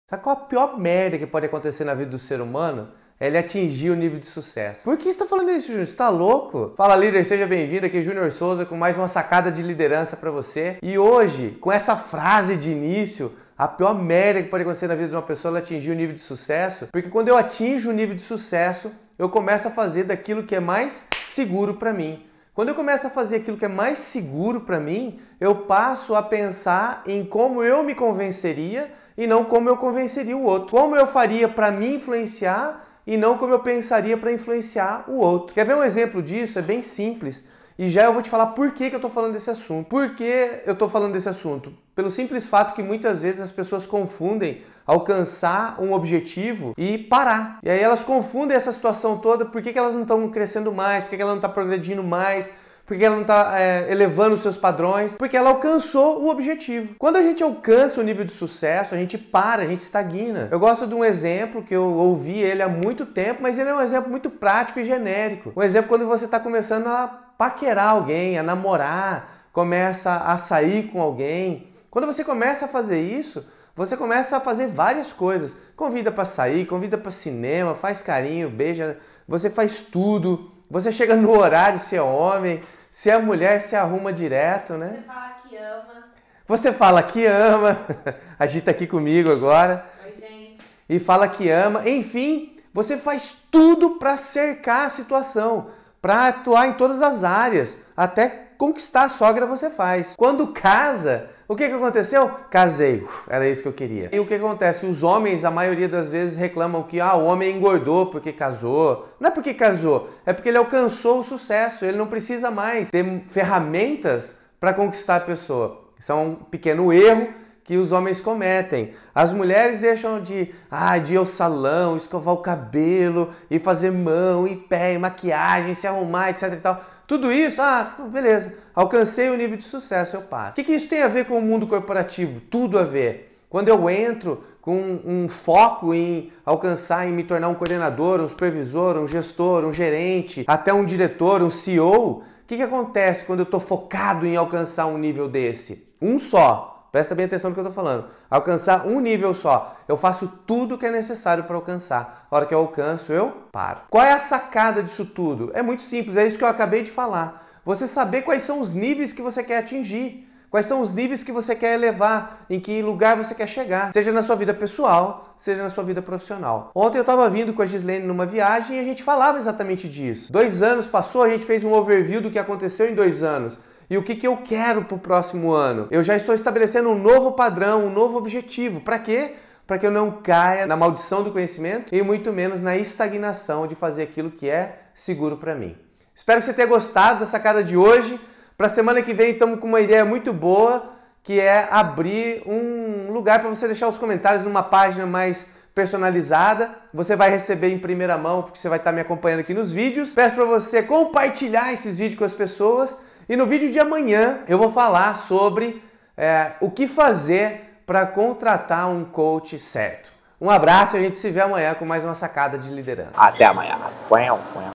A Maior "Merda" que Pode Acontecer na Vida de Um Líder #112
Neste vídeo eu quero compartilhar A Maior "Merda" que Pode Acontecer na Vida de Um Líder, e sabe qual é?

É ele atingir o sucesso, o objetivo.
Como assim?
Isso mesmo, quando atingimos o sucesso, temos uma tendência a começar a operar na zona de segurança, uma vez que agora eu estou onde queria.

Assista o vídeo e deixe seu comentário.